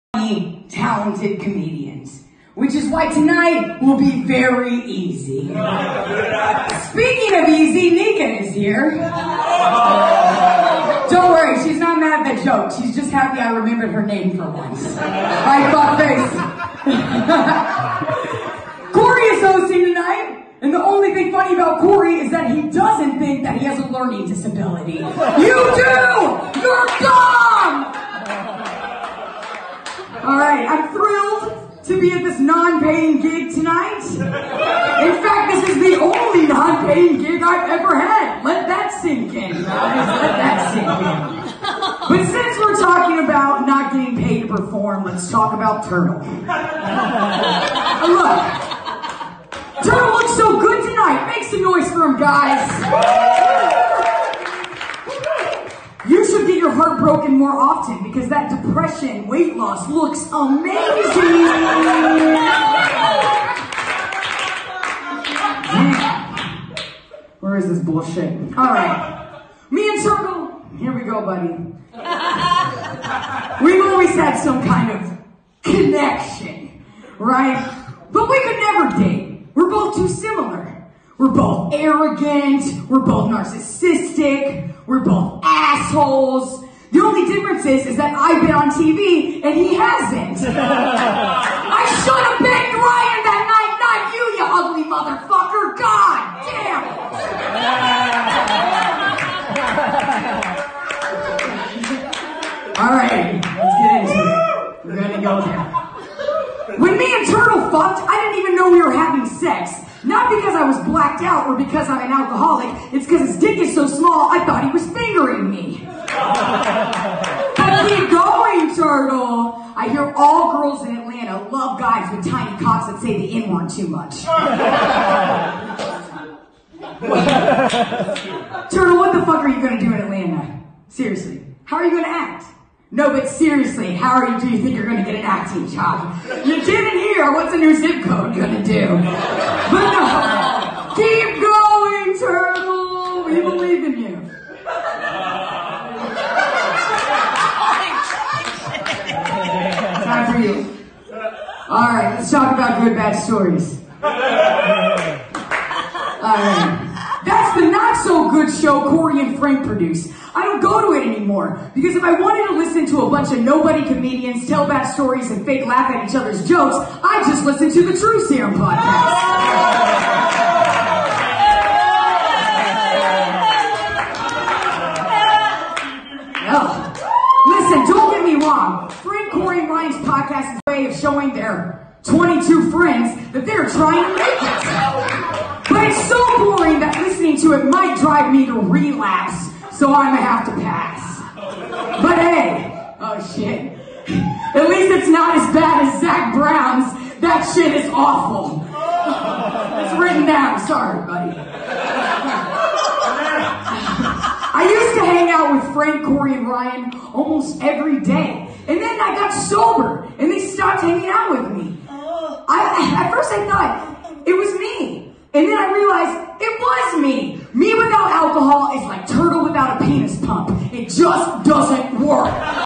Kay Day roasts comic Frank 'Turtle' Brenton
Kay Day at the Roast of Frank Brenton at The Peoples Improv Theater.